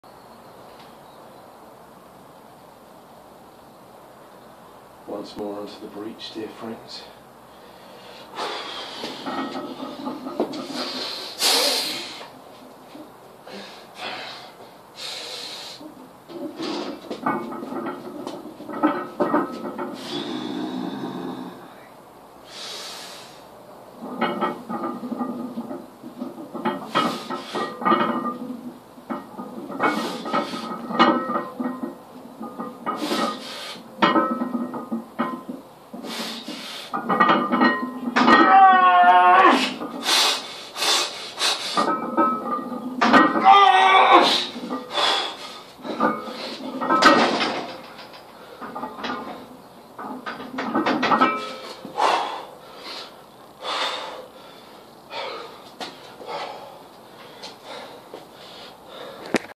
Squats 170kg set 6 of 6
Upper back and hamstrings had given in by this point but reps are still quite snappy.